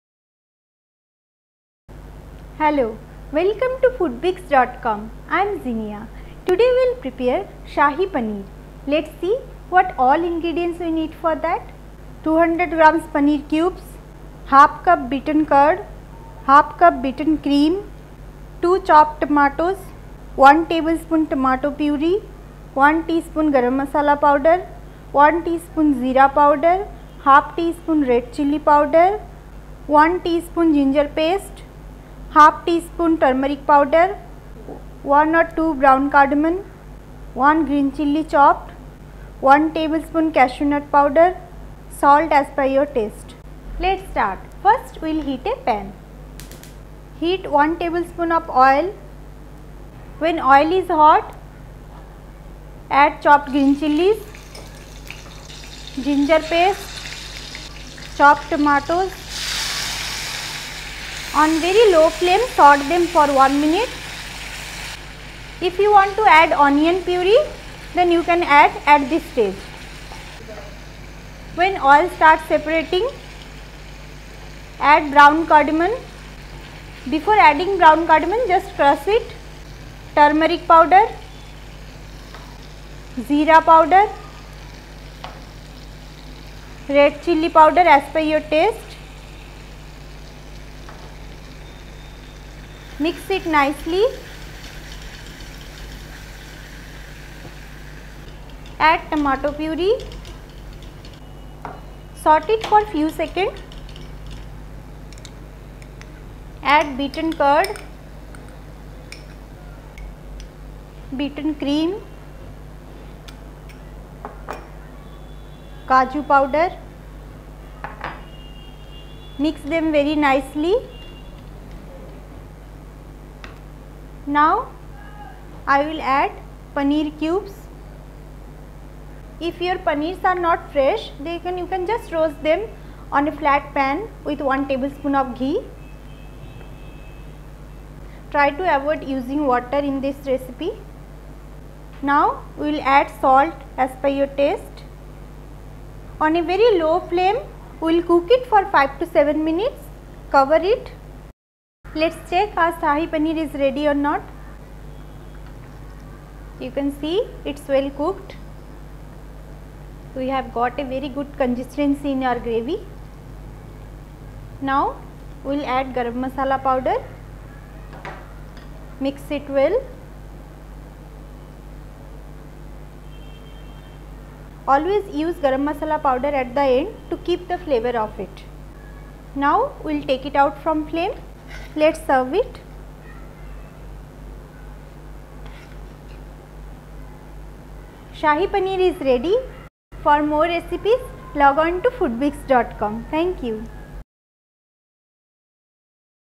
Shahi Paneer recipe Video - How to make Shahi Paneer Recipe
- Learn how to make/prepare Shahi Paneer by following this easy recipe